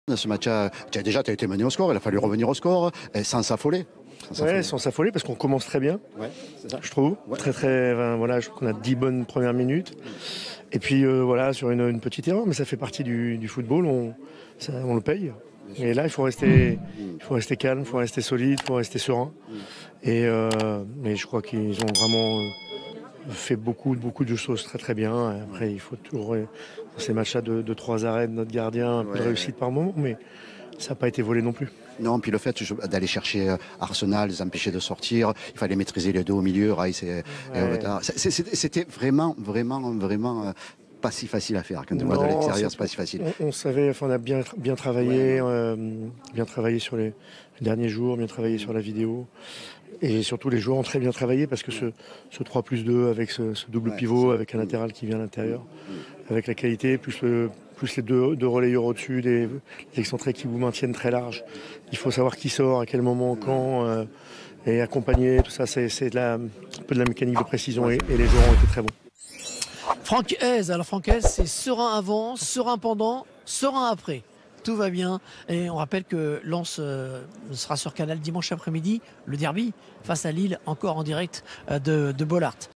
La réaction de Franck Haise après la victoire face à Arsenal " Je veux LDC " !
Ligue des champions. Lens réalise l’exploit en renversant Arsenal et prend la tête du groupe B
Le RC Lens a battu Arsenal au Stade Bollaert-Delelis pour la 2e journée de la Ligue des champions (2-1), ce mardi 3 octobre. Un succès de prestige pour les Sang et Or qui ont fait preuve d’une solidité exemplaire pour contrecarrer les plans des Gunners. Avec cette victoire, Lens prend seul la tête du groupe B.